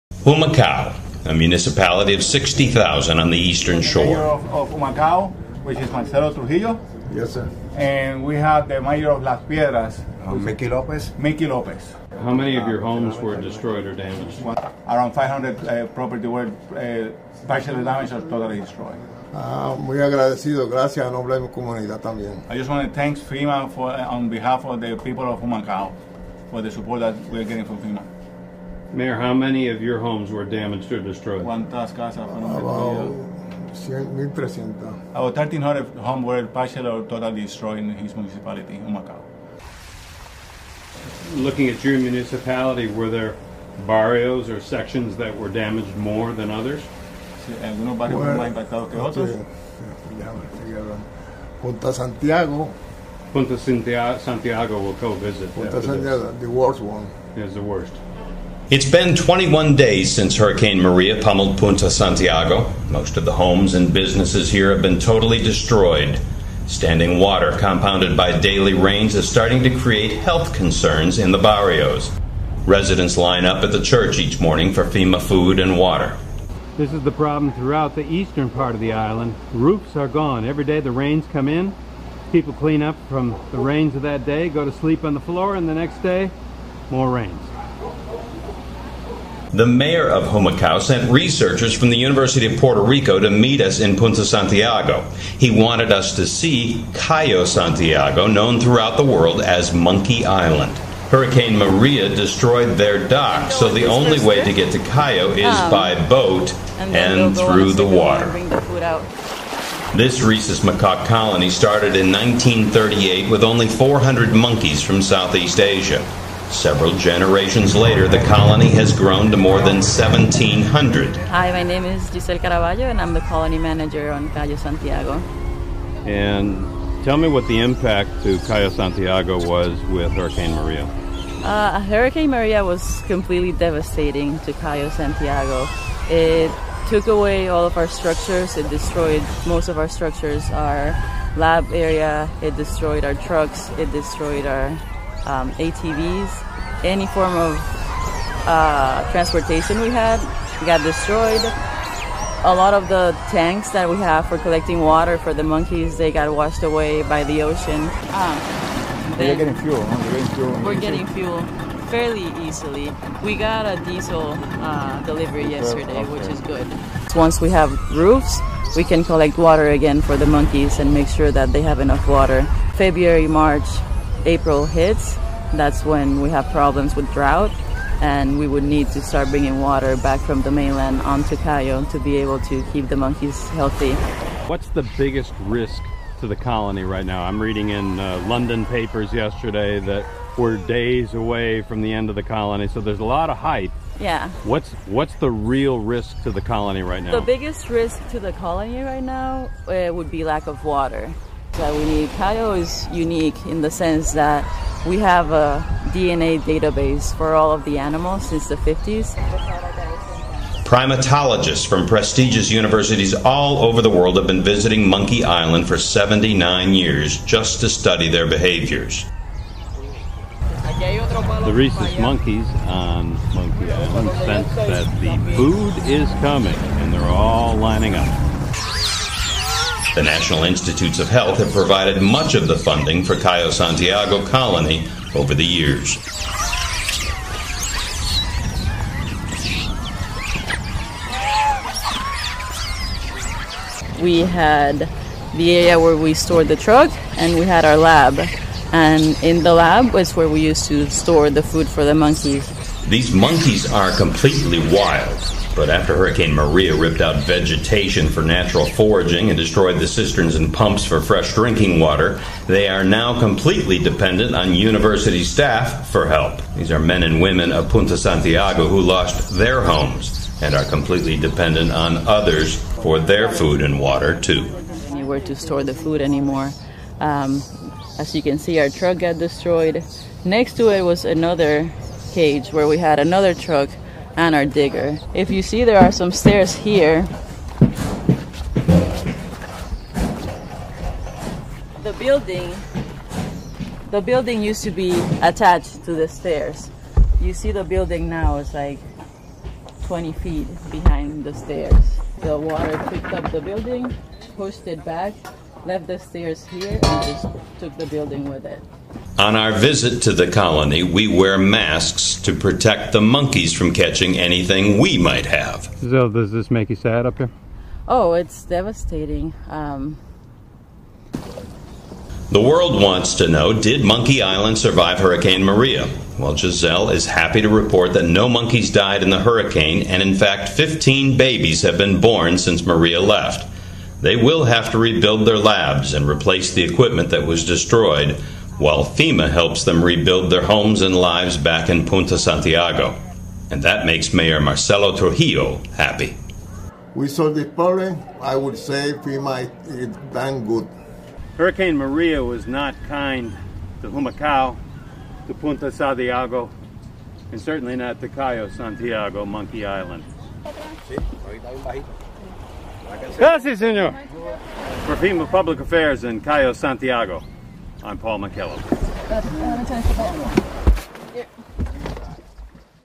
Monkey Island - Humacao - Punta Santiago
Mayor Marcelo Trujillo of Humacao, Puerto Rico discusses the worst hit barrio in his municipality from Hurricane Maria. FEMA staff is met by University of Puerto Rico researchers and guided to the world famous "Monkey Island" an NIH-funded rhesus macaque wild monkey colony on the island of Cayo Santiago. The research labs and facilities were destroyed but the monkeys are doing well.